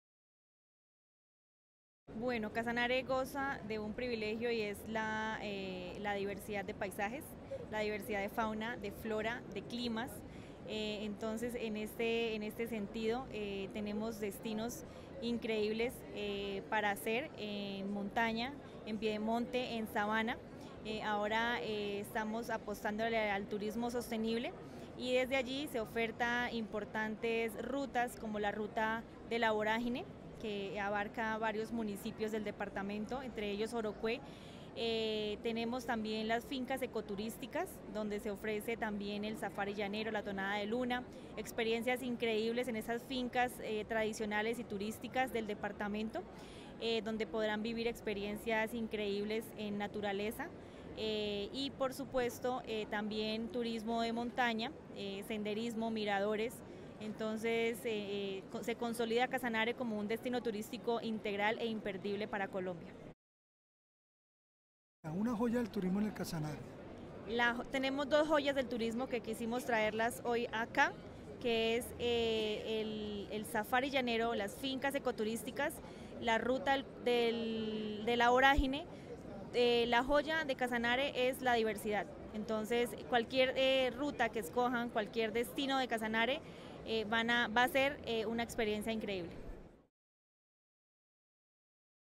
Casanare en la VitrinaANATO2024
Esta es la oferta que ell departamento del Casanare trajo a la VitrinaANATO2024.